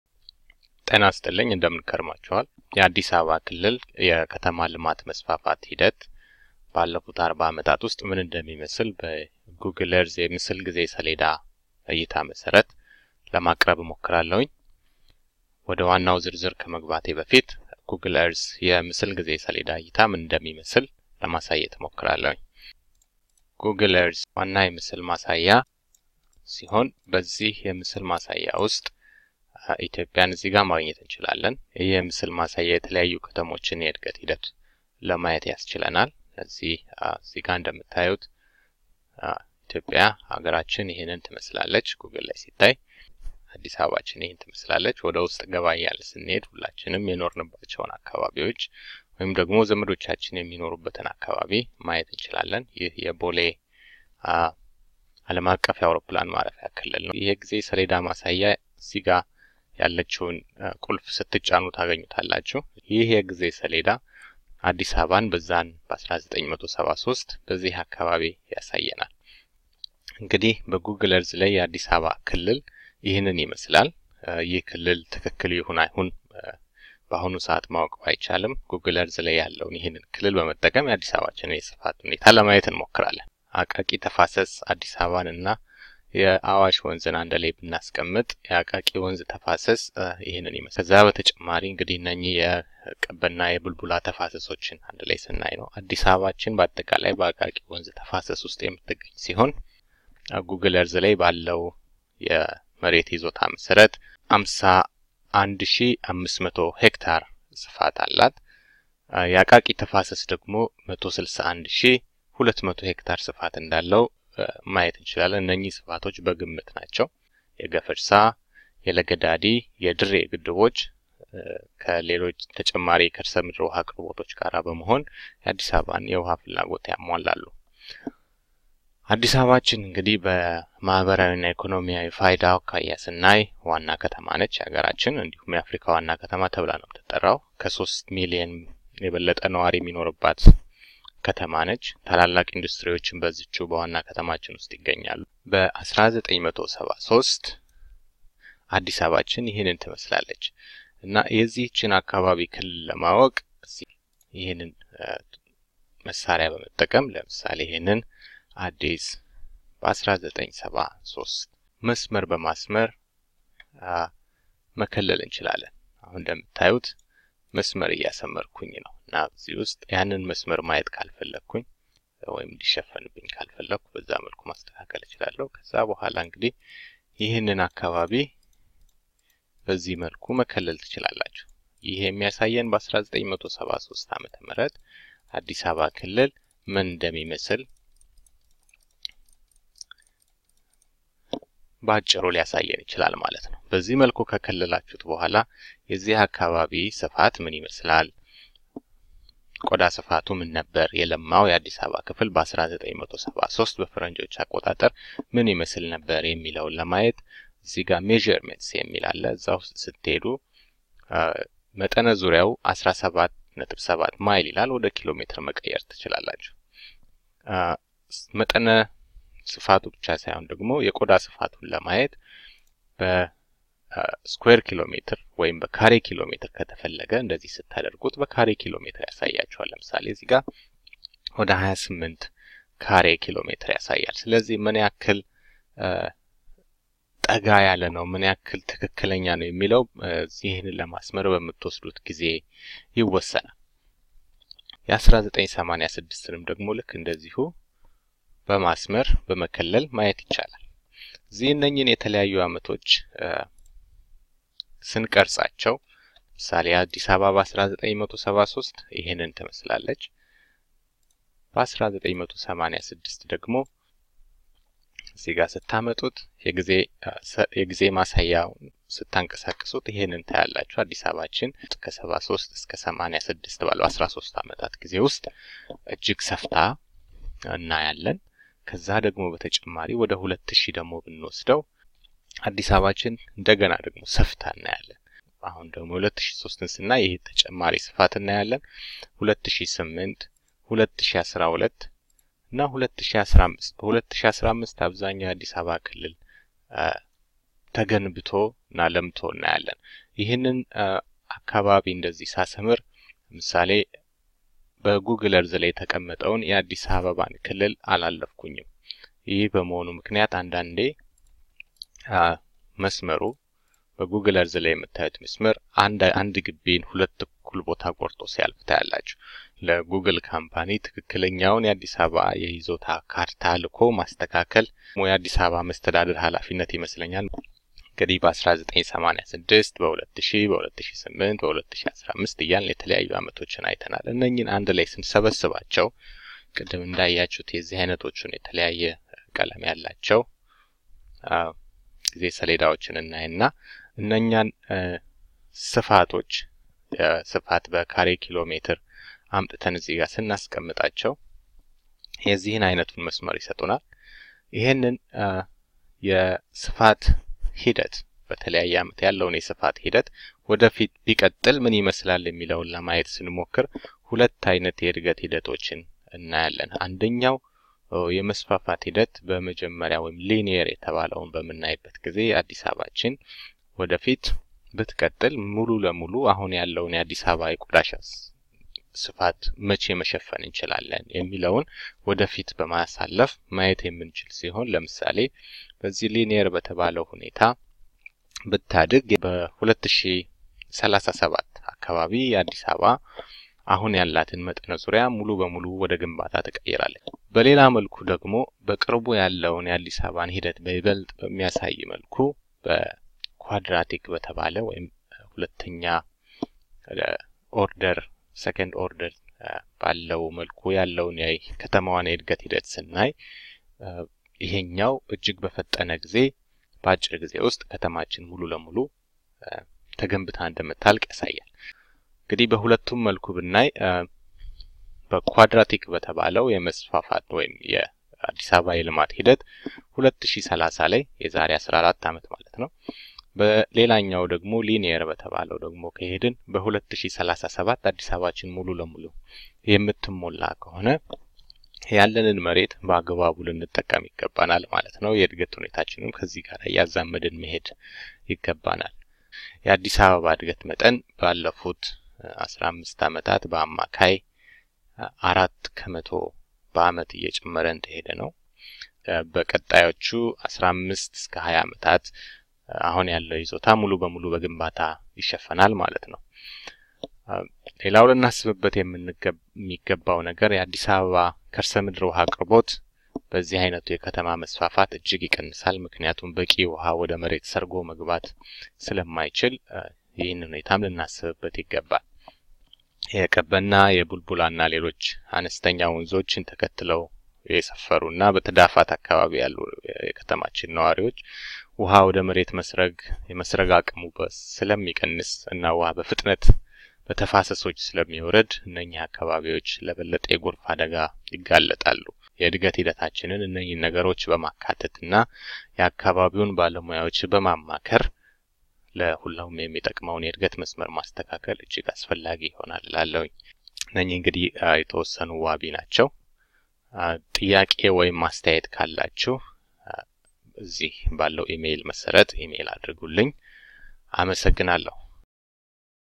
Addis Ababa Urban Expansion (1973 - 2015)
This short presentation is intended to show the urban area expansion in Addis Ababa, Ethiopia from 1973 to 2015 using Google Earth.